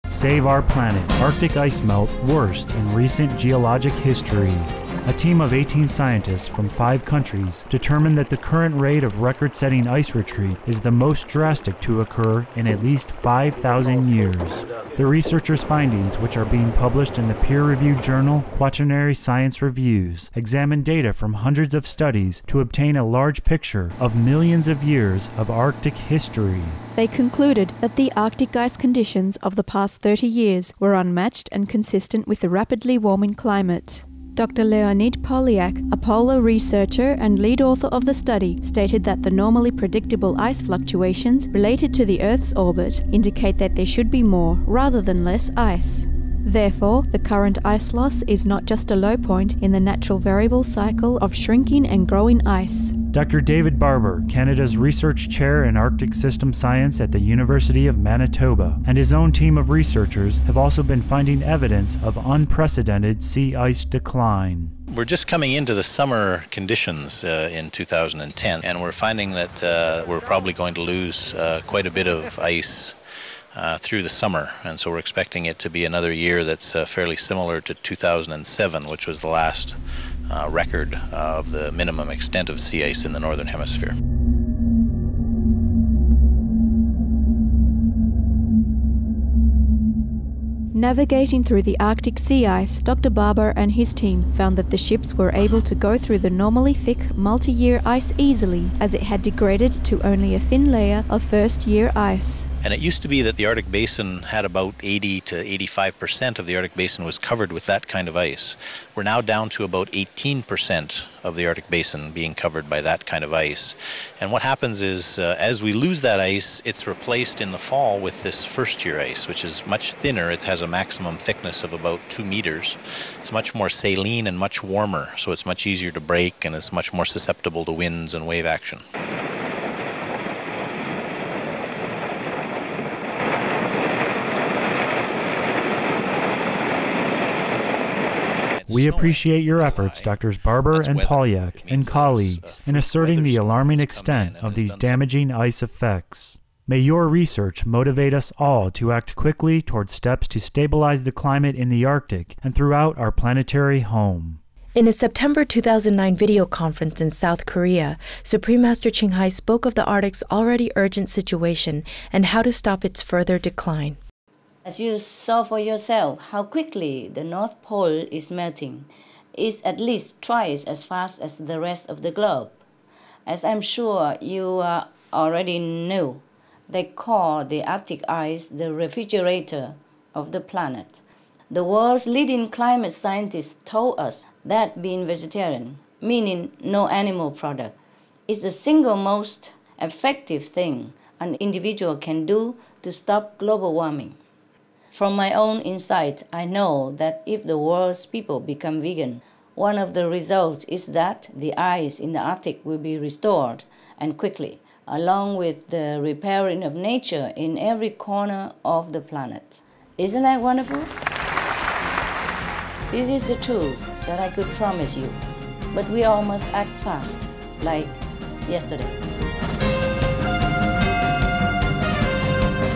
SAVE OUR PLANET - Arctic ice at low point compared to recent geologic history
SAVE OUR PLANET... Arctic ice melt worst in recent geologic history. A team of 18 scientists from five countries determined that the current rate of record-setting ice retreat is the most drastic to occur in at least 5,000 years. The researchers' findings, which are being published in the peer-reviewed journal Quaternary Science Reviews, examined data from hundreds of studies to obtain a large picture of millions of years of Arctic history. They concluded that the Arctic ice conditions of the past 30 years were unmatched and consistent with the rapidly warming climate. Dr. Leonid Polyak, a polar researcher and lead author of the study, stated that the normally predictable ice fluctuations related to the Earth's orbit indicate that there should be more, rather than less ice. Therefore, the current ice loss is not just a low point in the natural variable cycle of shrinking and growing ice.

Dr. David Barber, Canada's Research Chair in Arctic System Science at the University of Manitoba and his own team of researchers has also been finding evidence of unprecedented sea ice decline.

David Barber (M): We're just coming into the summer conditions in 2010, and we're finding that we're probably going to lose quite a bit of ice through the summer. And so we're expecting it to be another year that's fairly similar to 2007, which was the last record of the minimum extent of sea ice in the northern hemisphere.

VOICE: Navigating through the Arctic sea ice, Dr. Barber and his team found that the ships were able to go through the normally thick multi-year ice easily, as it had degraded to only a thin layer of first-year ice.

David Barber (M): It used to be that about 80 to 85% of the Arctic basin was covered with that kind of ice. We're now down to about 18% of the Arctic basin being covered by that kind of ice. And what happens is as we lose that ice, it's replaced in the fall with this first-year ice which is much thinner. It has a maximum thickness of about 2 meters. It's much more, say, lean, and much warmer, so it's much easier to break and it's much more susceptible to winds and wave action.

VOICE: We appreciate your efforts, Drs. Barber and Polyak and colleagues, in asserting the alarming extent of these damaging ice effects. May your research motivate us all to act quickly toward steps to stabilize the climate in the Arctic and throughout our planetary home.

In a September 2009 videoconference in South Korea Supreme Master Ching Hai spoke of the Arctic's already urgent situation and how to stop its further decline.

Videoconference with Supreme Master Ching Hai
Jeju Island, South Korea -- September 21, 2009

Supreme Master Ching Hai: As you saw for yourself how quickly the North Pole is melting - it's at least twice as fast as the rest of the globe. As I'm sure you already knew, they call the Arctic ice the refrigerator of the planet.

The world's leading climate scientists told us that being vegetarian - meaning no animal products - is the single most effective thing an individual can do to stop global warming.

From my own insight, I know that if the world's people become vegan, one of the results is that the ice in the Arctic will be restored, and quickly, along with the repairing of nature in every corner of the planet. Isn't that wonderful? This is the truth that I could promise you, but we all must act fast, like, yesterday.